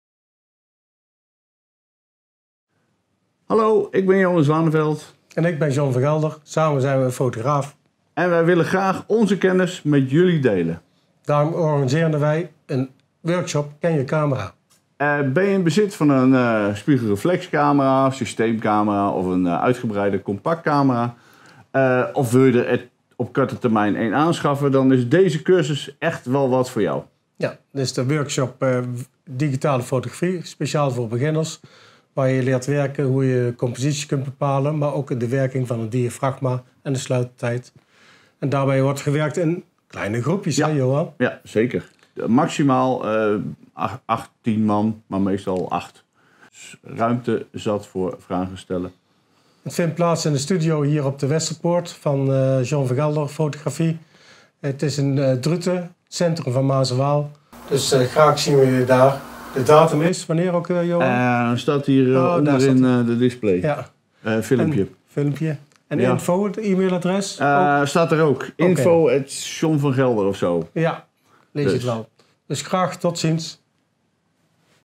Promofilmpje ken je camera 11 april
In deze workshop van 1 avond  behandel ik alle in's en out's van jouw/uw camera. Dus echt  de basis van alle fotografie, tip and tricks.